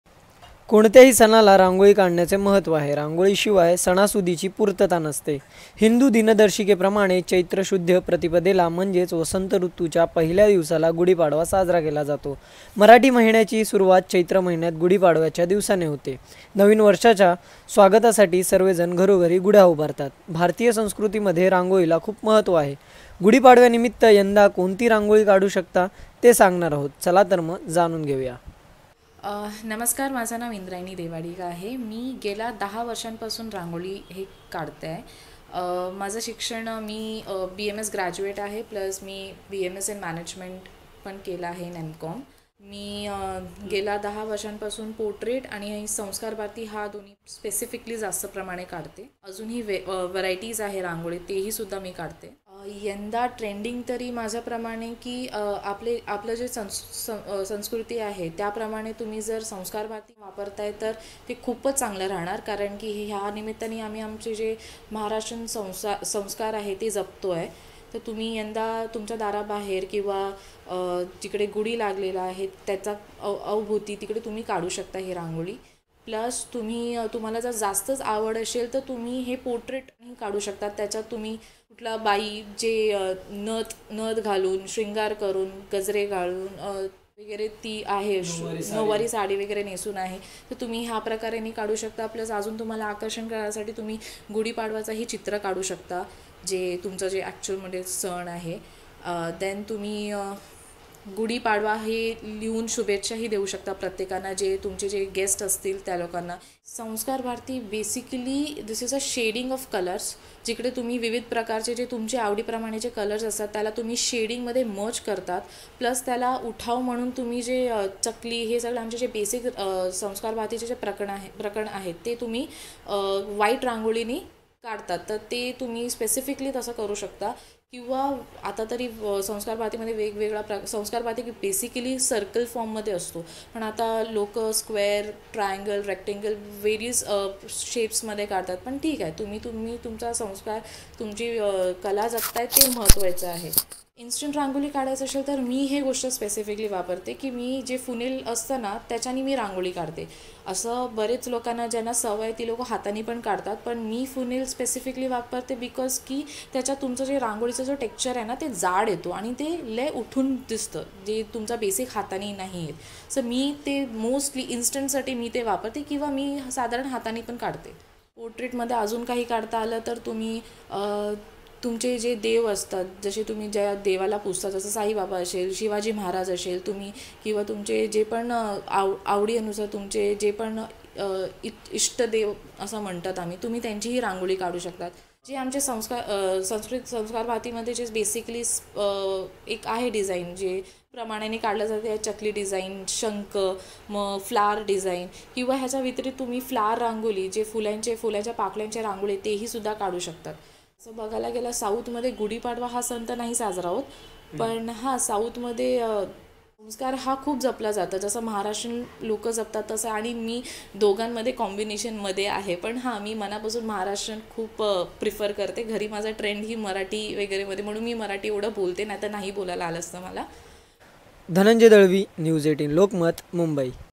Gudi Padwa 2023 | यंदा गुढीपाडव्याला दारापुढे काढा झक्कास रांगोळी | Rangoli Designs
Gudi Padwa 2023 Rangoli Designs  : गुढीपाडव्यानिमित्त यंदा तुम्ही गुढी पुढे तसेच दाराबाहेर कोणती रंगोळी काढू शकता जाणून घ्या.